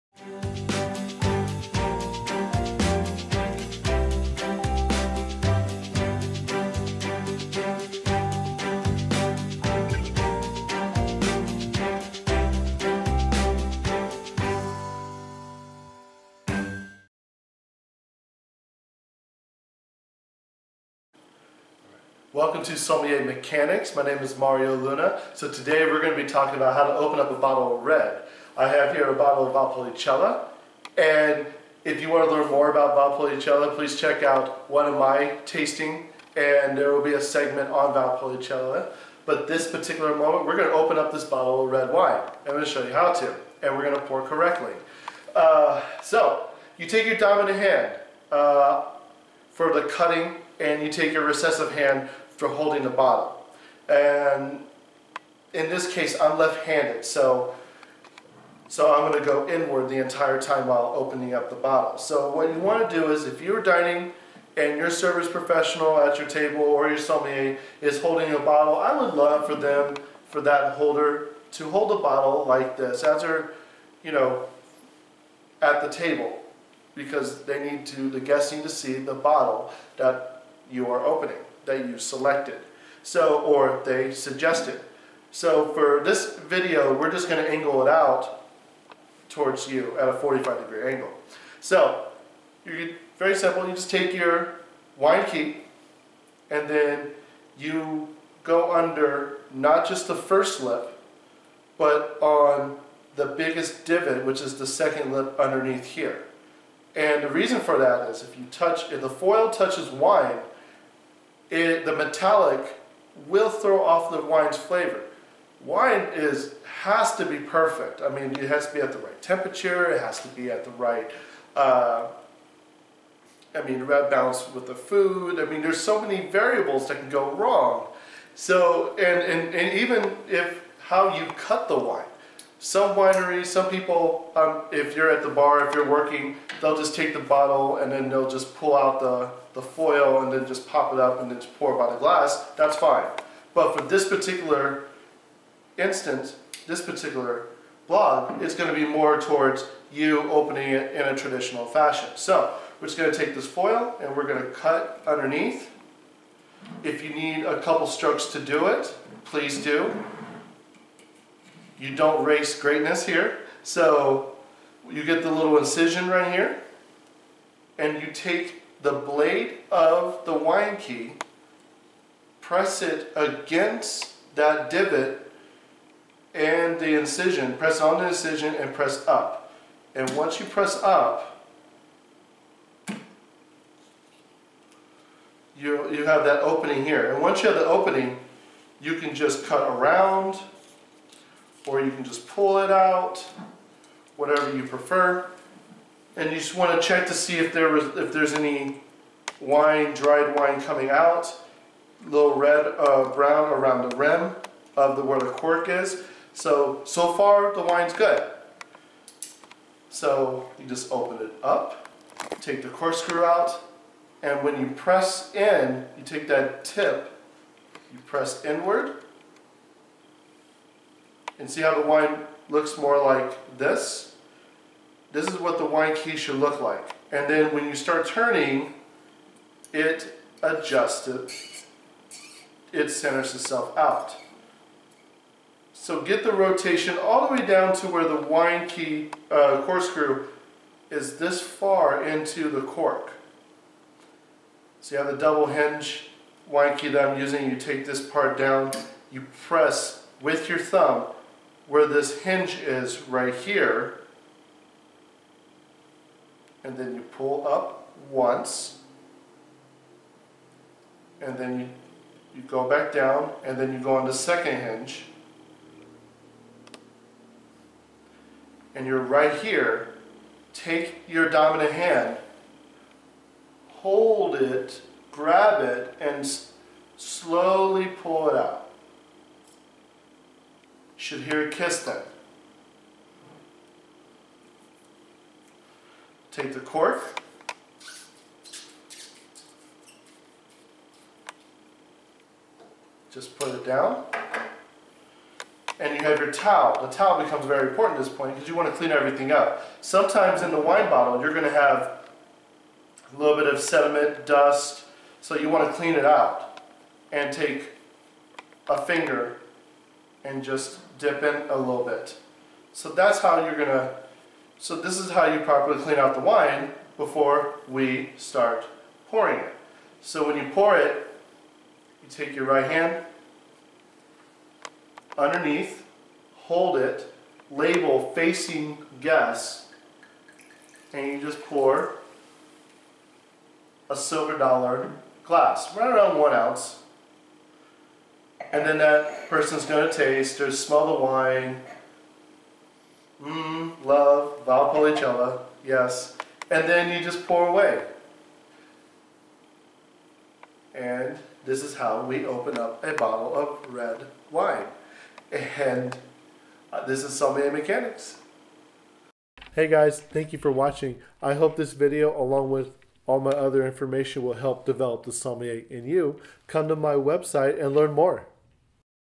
Sommelier Mechanics: How to Open a Bottle of Red Wine HD
In this edition of Sommelier Mechanics, Mario takes you through the process of how to effectively open a bottle of wine. It seems easy...

Please enjoy this Vlog and all other food, wine, and spirits education and "Develop to Sommelier in You!"